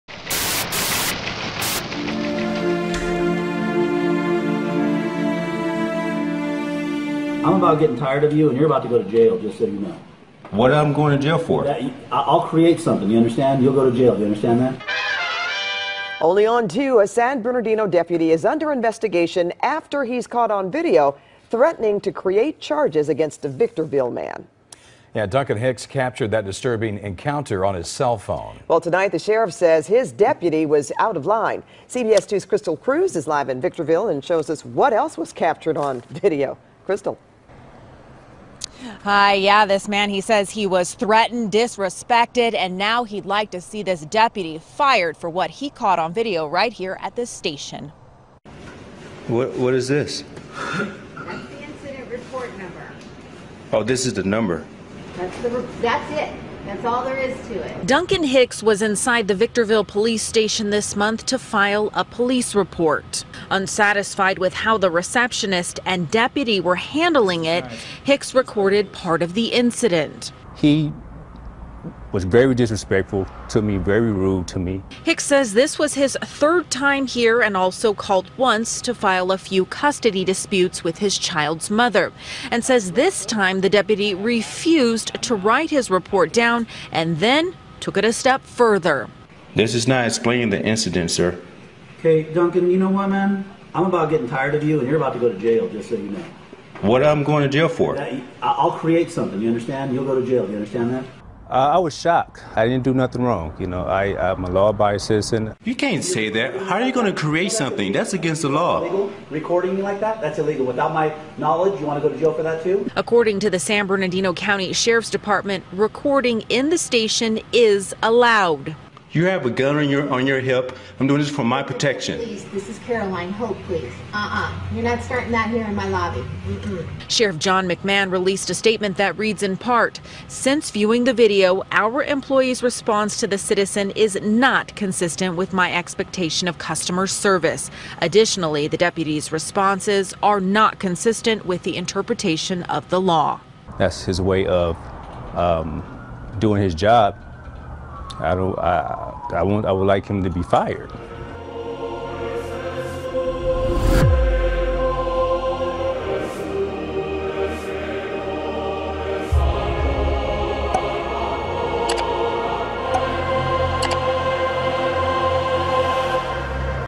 Self Appointed Police Officer Lawmaker
How much does a Deputy Sheriff make in San Bernardino, CA? The average Deputy Sheriff salary in San Bernardino, CA is $123,360 as of January 26, 2023, but the range typically falls between $116,490 and $130,860. Qualified Immunity must be Banned!

We the PEOPLE need Law Enforcement that is a member of our Community - Not more Blue Line Perverts.

Amnesty International